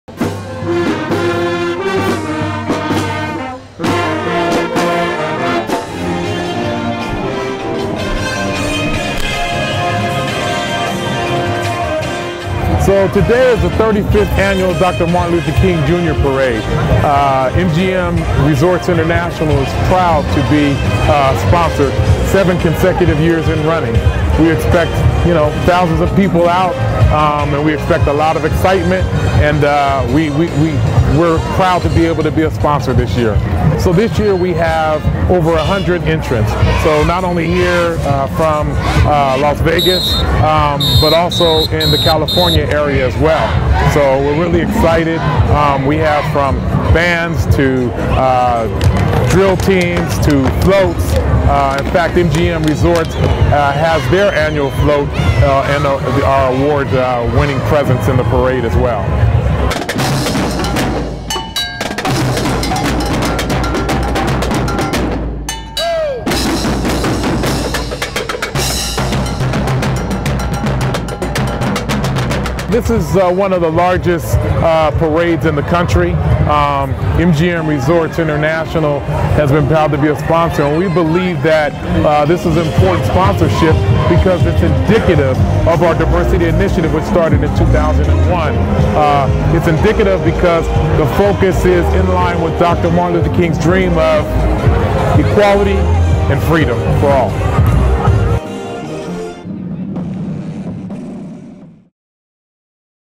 MLK Day Parade celebrates 35th year
The 35th annual Martin Luther King Day Parade makes it’s way down Fourth Street in Las Vegas.